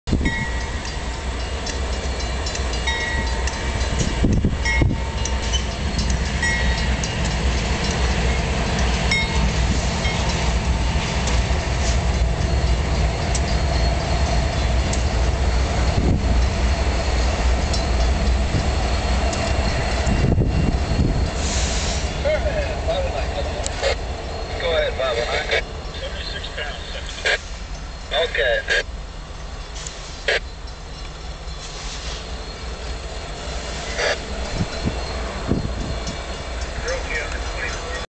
cn 393 ? leaving sarnia for the usa
canadian cab dash 9 - 2506  ex up dash 8-2123, sd75 -5663 westbound  sept 11/10